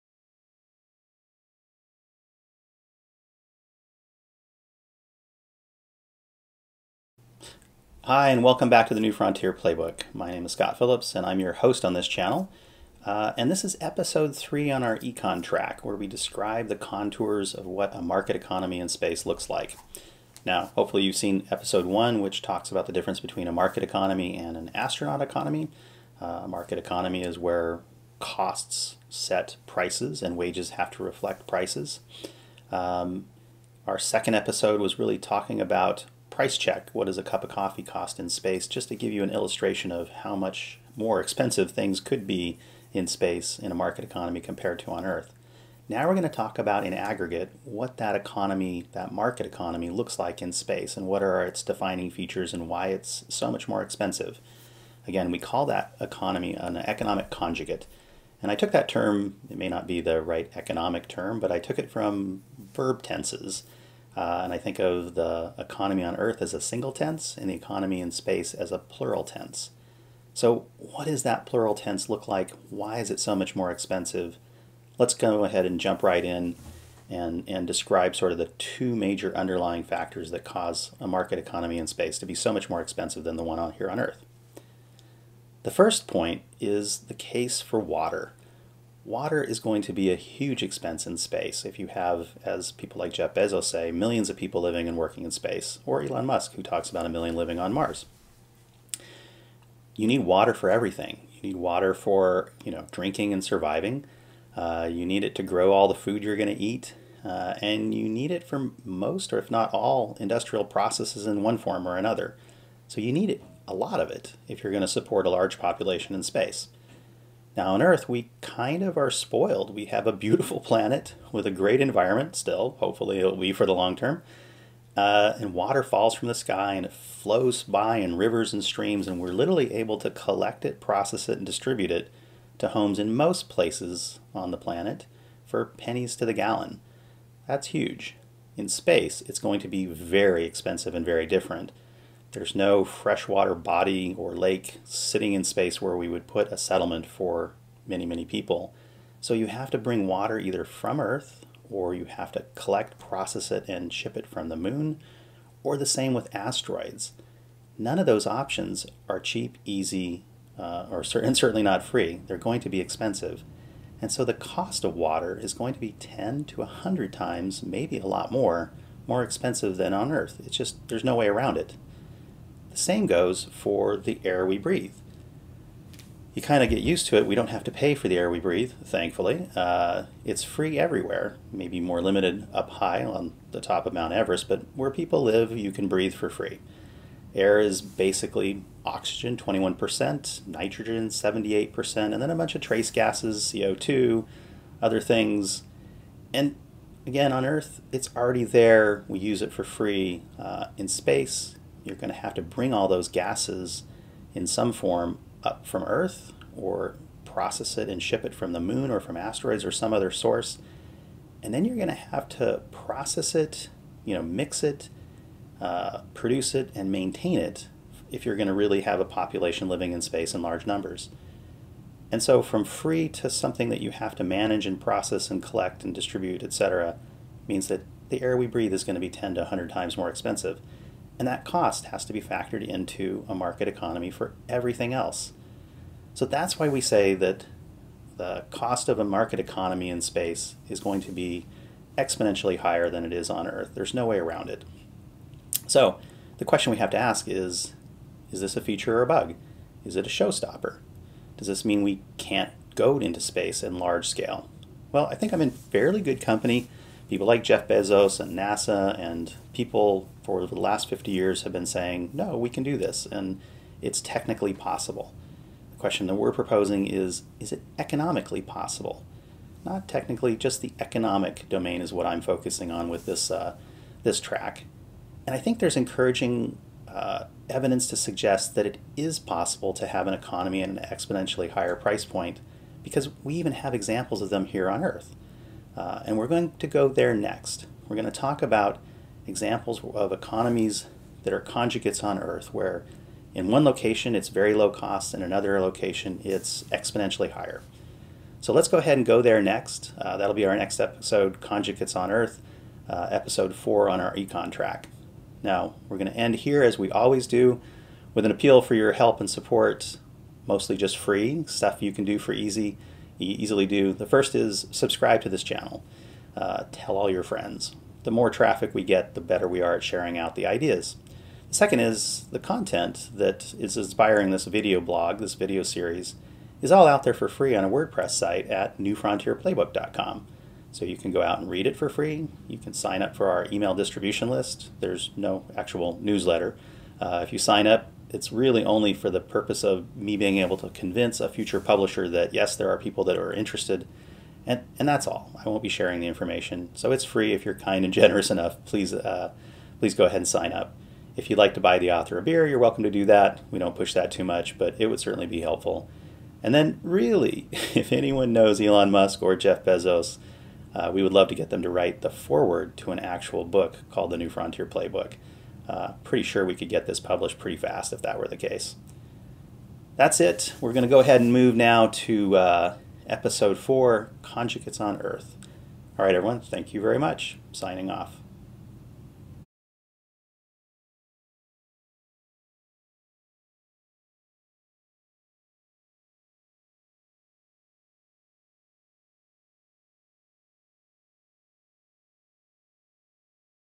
ECON 3: Economic Conjugate
This third episode in the The New Frontier Playbook's Economics track describes the underlying factors that make a market economy in space exponentially higher cost than one on Earth, an economy we refer to as an Economic Conjugate.